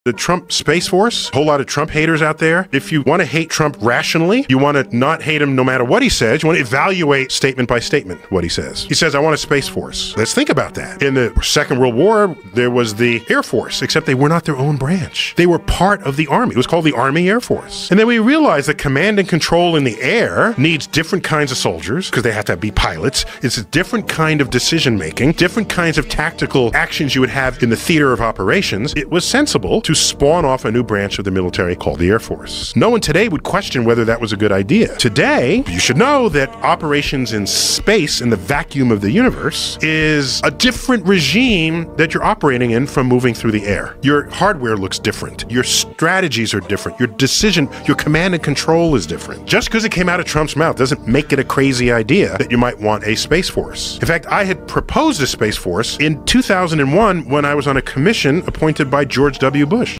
Why Space Force Isn’t as Crazy as It Sounds w/ Neil deGrasse Tyson
The Trump Space Force sounds crazy — until you look at modern warfare.